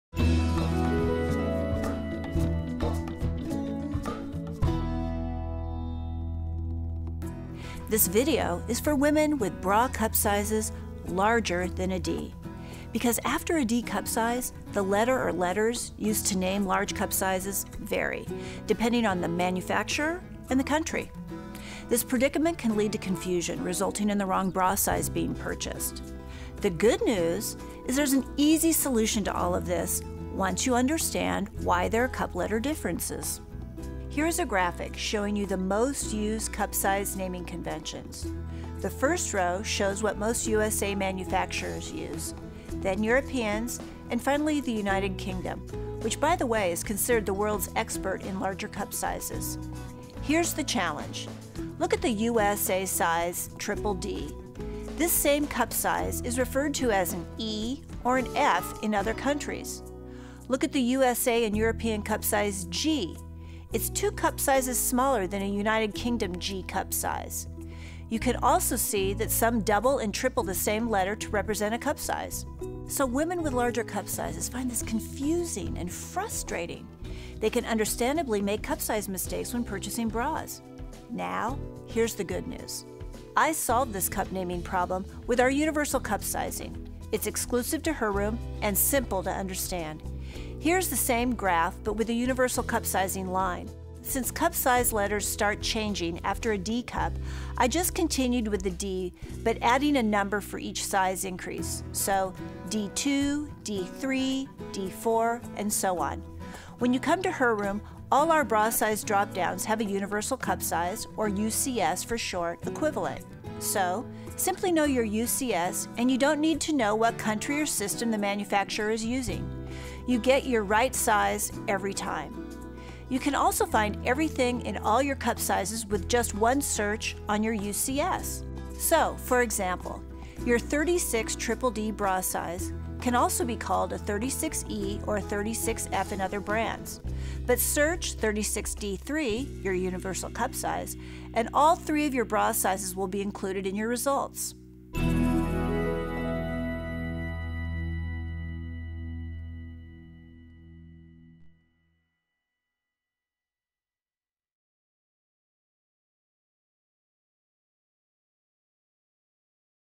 How to Measure Bra Size: The Difference Between USA, EU, & UK Bra Sizes
If you are larger than a D cup, you probably know that your bra size changes depending on the brand you are buying from. In this video our expert and CEO, Tomima Edmark, explains how to find your size in all brands.